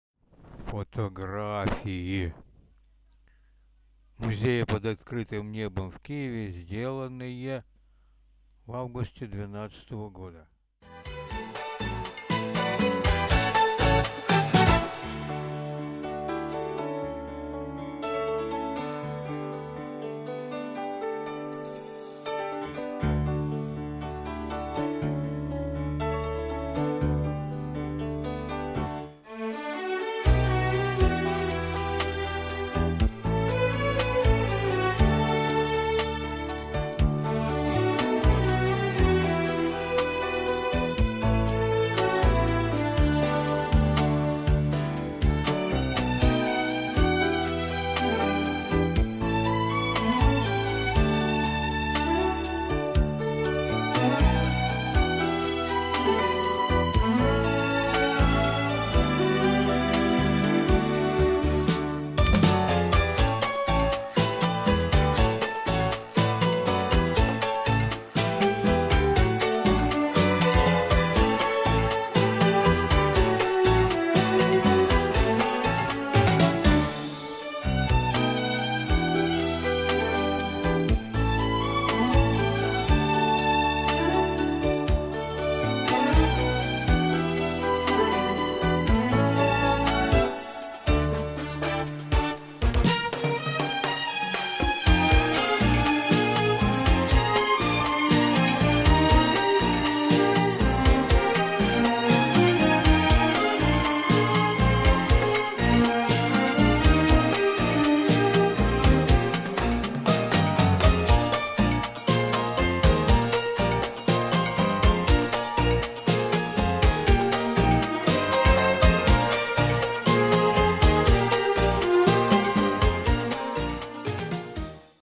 Музей в Пирогово
Ролик сделан программой ФотоШоу ПРО. Картинки музея под открытым небом из Пирогово под Киевом и виды левого берега Днепра с правого. Музыка от Paul Morriot.
    UMG